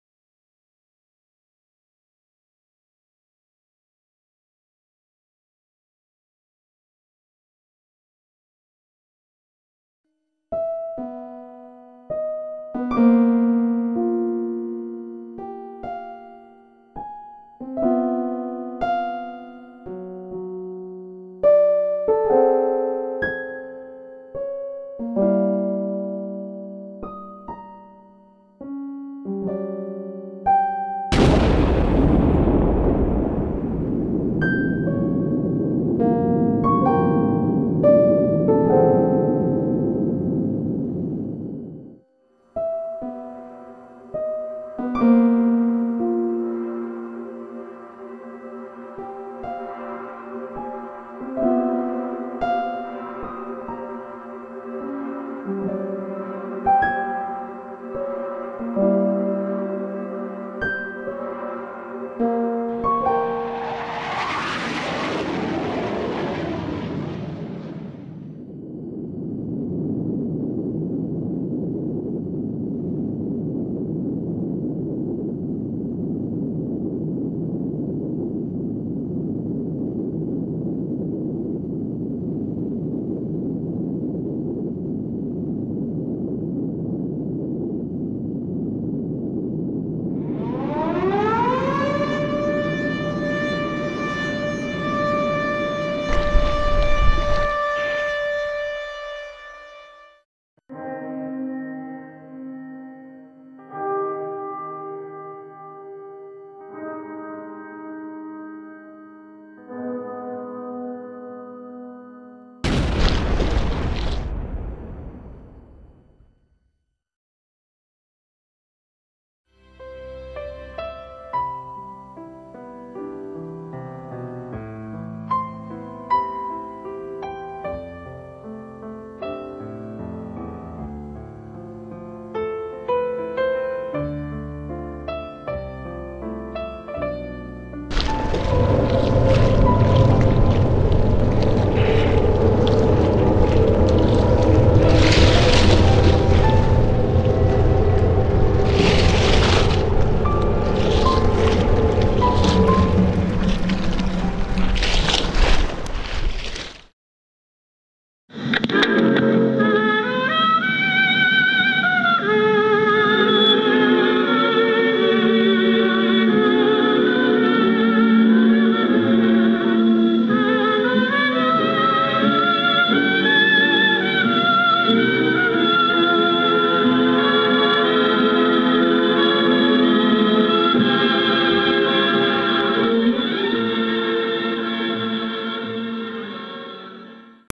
Beyond the wall
A short film made for a college project which had to be based around the Cold War. It's a simple love story mixed with the paranoia of an atomic holocaust.